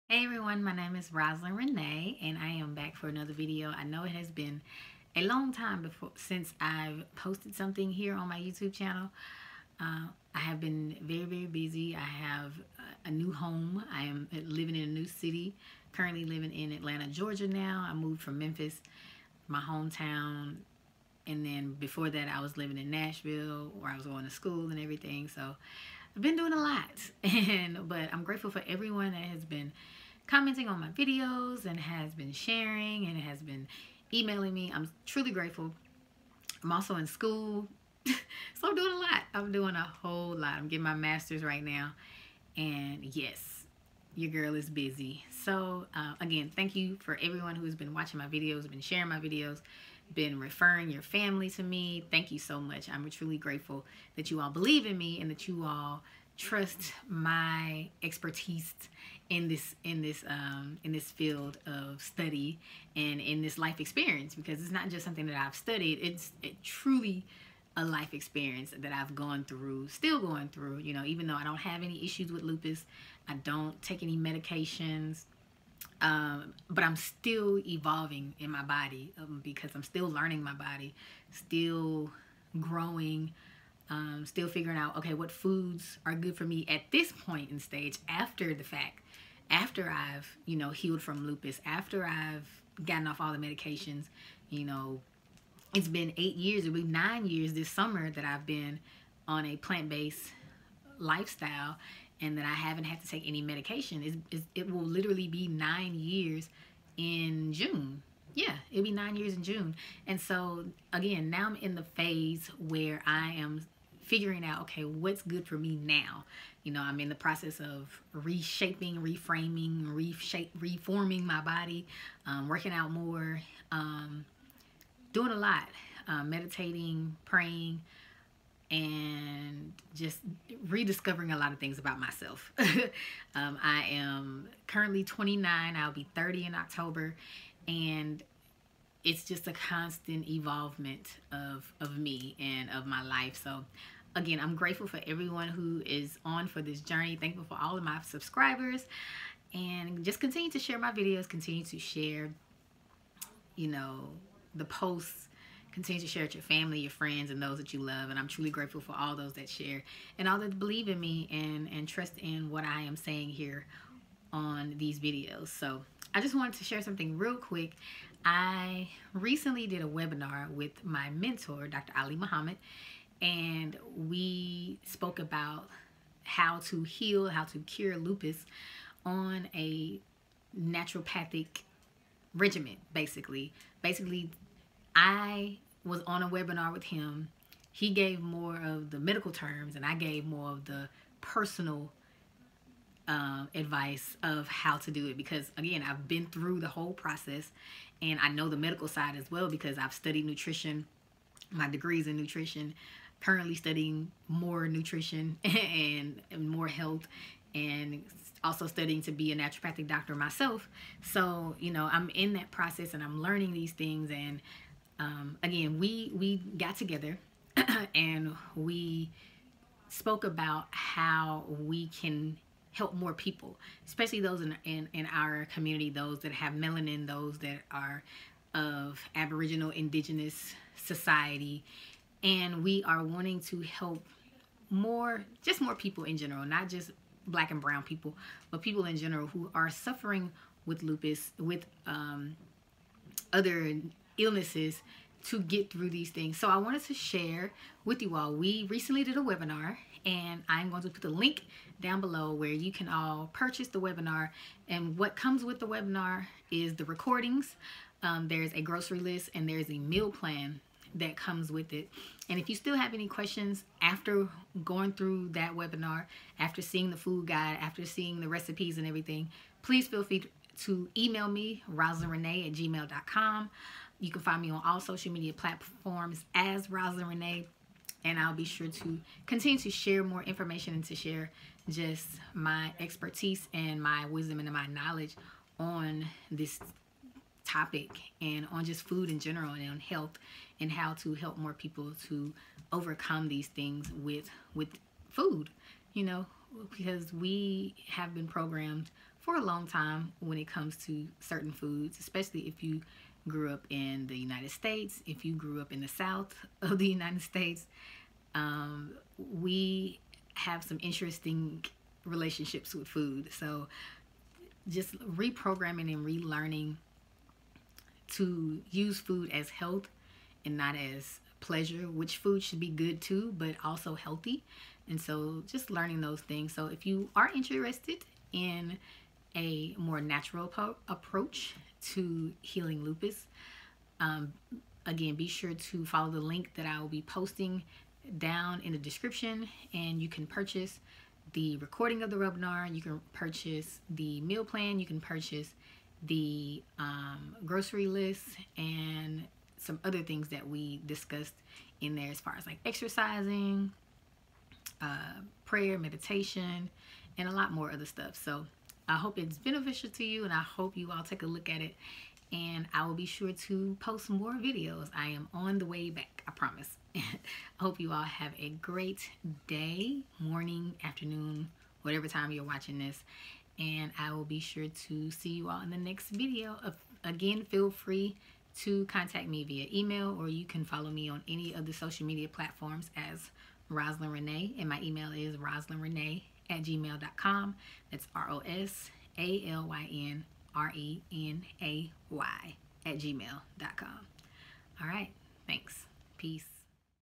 Lupus webinar
I want to first say thank you to everyone that has watched, shared, and made comments on my videos. You all are appreciated. Below is a link to a webinar that myself and my mentor Dr Ali Muhammid did together. This webinar is filled with very valuable information and I know it will help many.